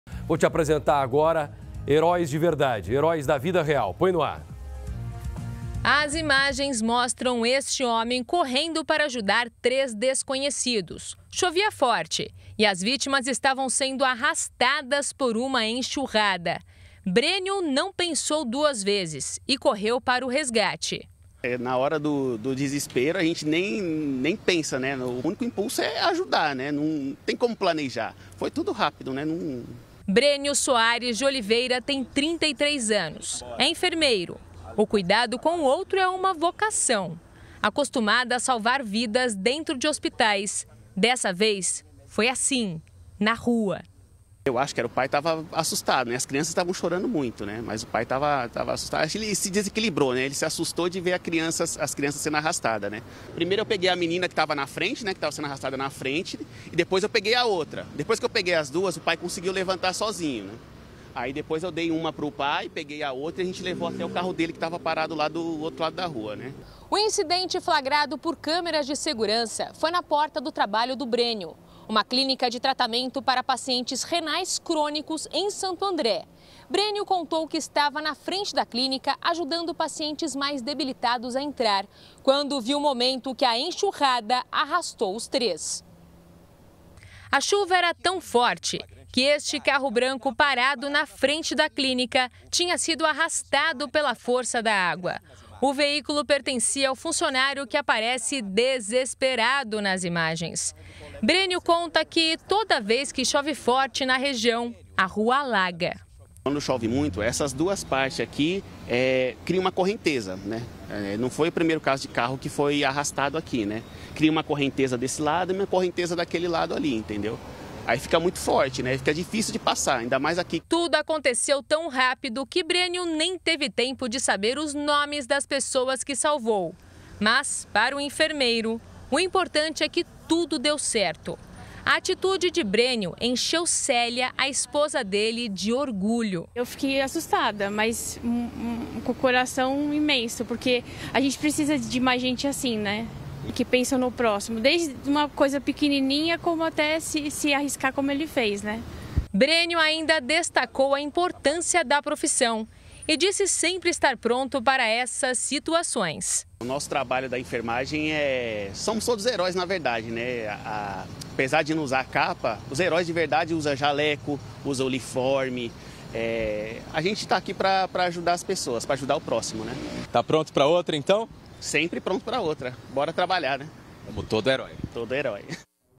Enfermeiro salva a vida de três pessoas arrastadas por enxurrada em São Paulo
Um enfermeiro salvou a vida de um homem e duas crianças arrastados pela força da enxurrada em São Paulo. Ele estava m frente à clínica onde trabalha quando percebeu que as três pessoas estavam em apuros e não pensou duas vezes em agir.